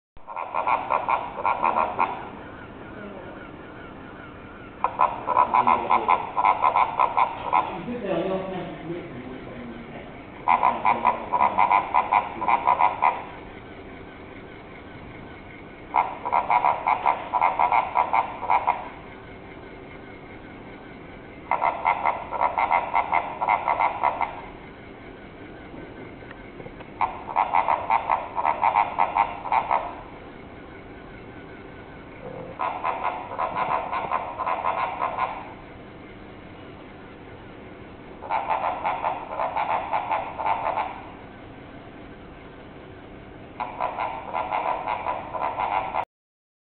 Pig in a Wig
Jason Byrne very kindly donated a Pig in a Wig - one of only FOUR IN THE WORLD - as a competition prize for ThreeWeeks, the Edinburgh Festival Newspaper.